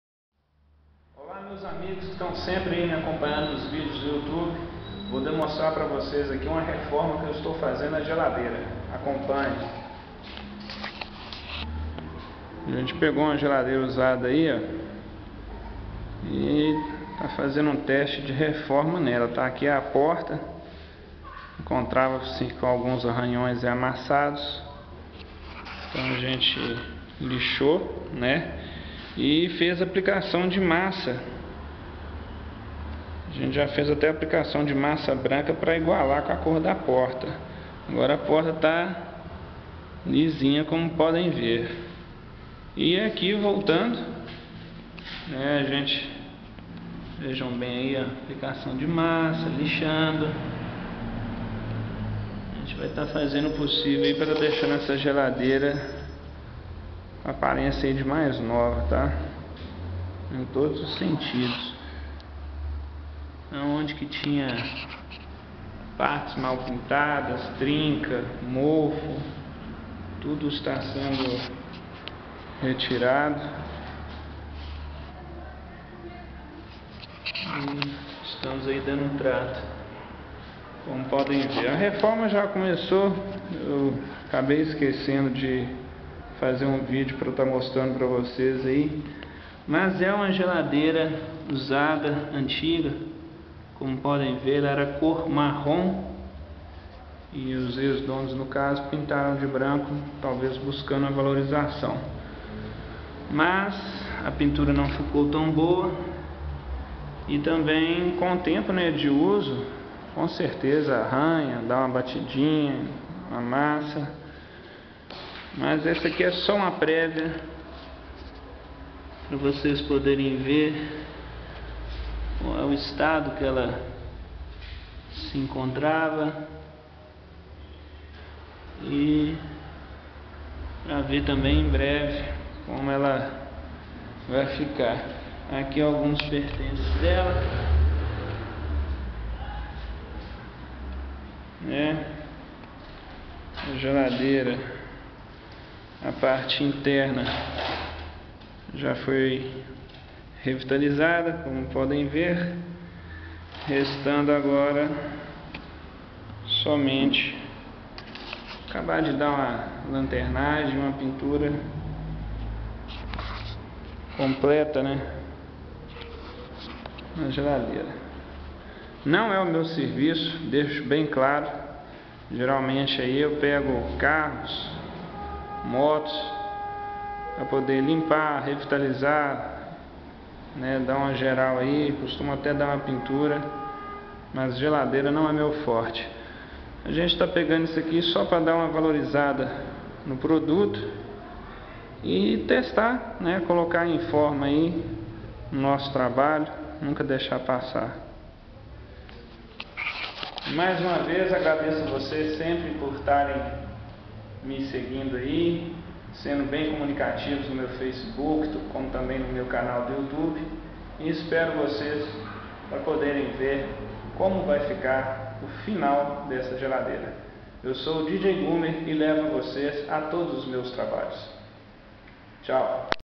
DJGummer reformando geladeira parte 1
DJGummer testando seus conhecimentos reformando e revitalisando uma geladeira, assistem e acompanha este trabalho artesanal por este primeiro video, conheça mais sobre DJGummer acessando o facebok